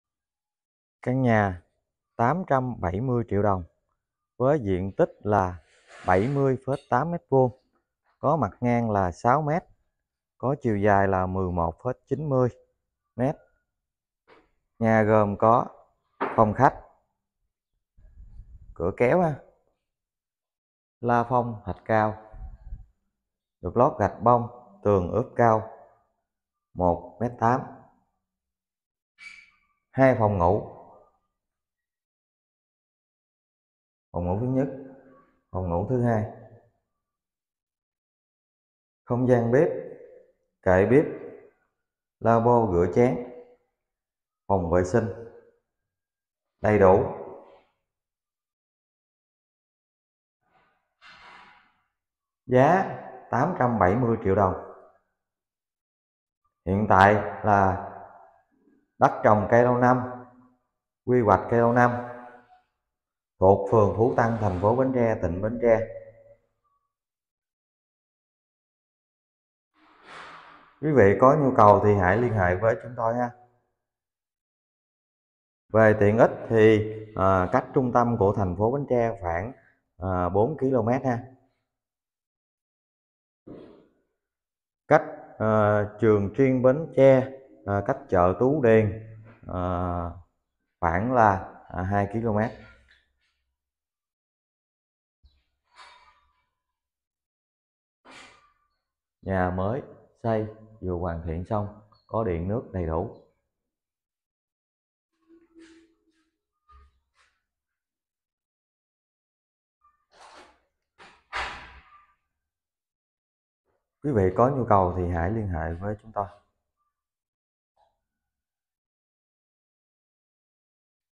(ĐÃ BÁN)Bán nhà mới 100% Phú Tân, TP Bến Tre| Nhà đất Bến Tre| Chợ Bến Tre
Bán nhà mới 100% Phú Tân, TP Bến Tre
Tổng dt: 71m2(6x12).
Đất CLN quy hoạch CLN.
Sổ thể hiện đường đi.
Hướng Bắc.
Nhà gồm 1 phòng khách, 2 phòng ngủ, bếp, wc
Vị trí: cách đường vành đai 50m, đường xe hơi
Tiện ích sẵn sổ, gần trung tâm TP Bến Tre, nhà mua về dọn vào ở ngay, gần trường học các cấp, gần chợ, có sẵn điện nước,…
Giá 870 triệu, thương lượng nhẹ